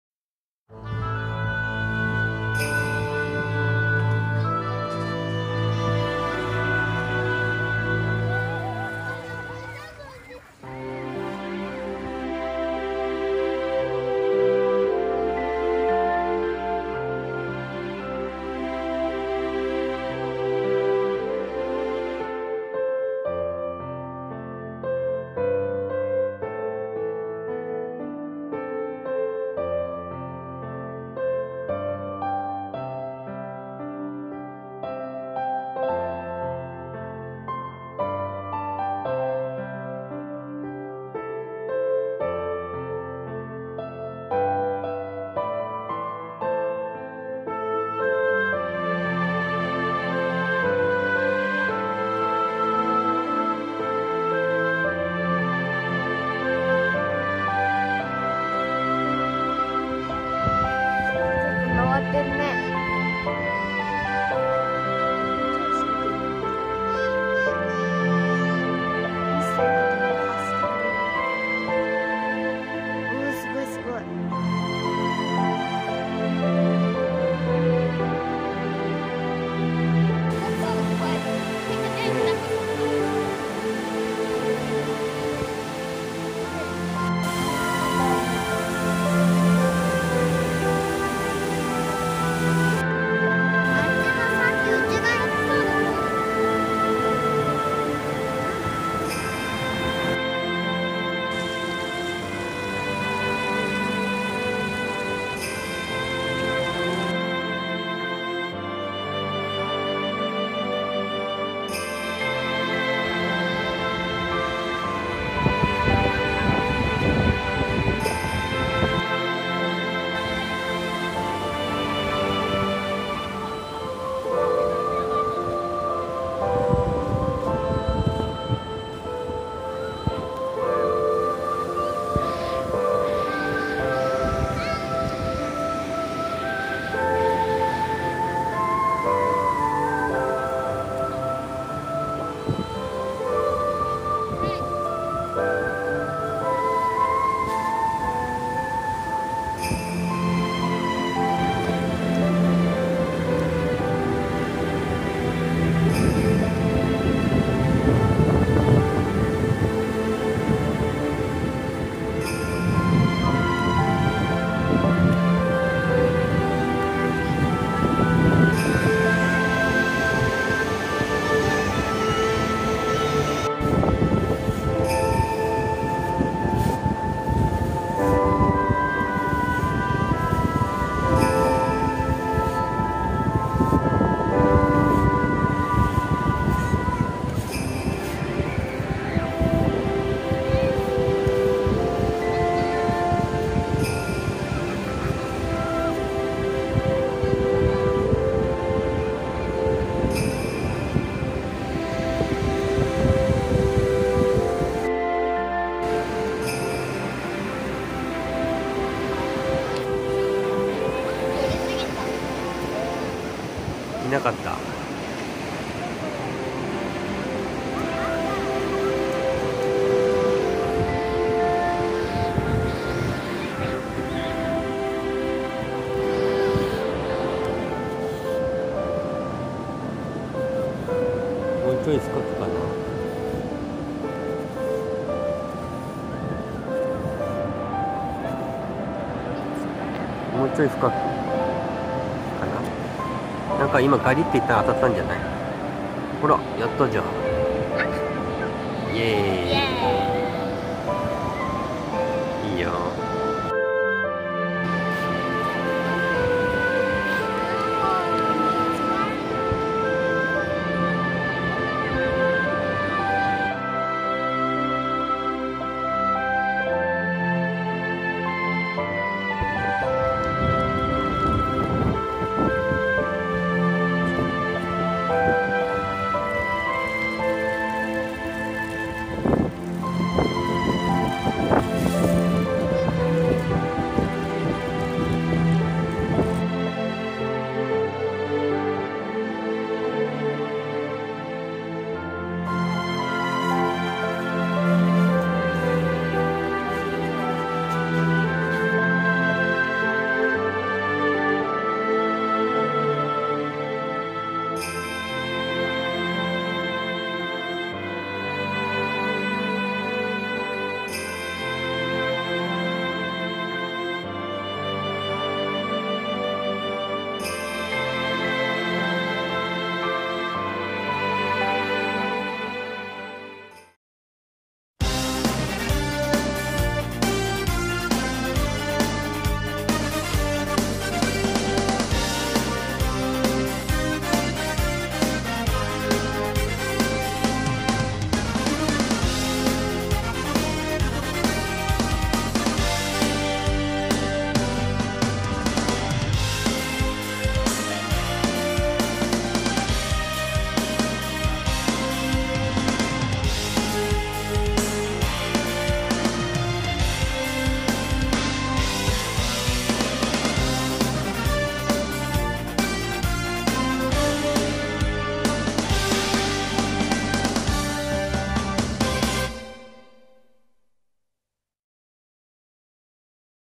[bushcraft33]ブッシュクラフトスキルをいかして鹿児島でアウトドア満喫旅行 北海道ブッシュクラフトシリーズ番外編
鹿児島にて、ブッシュクラフトスキルを活用しアウトドア満喫してきましたლ(❛ᴗ❛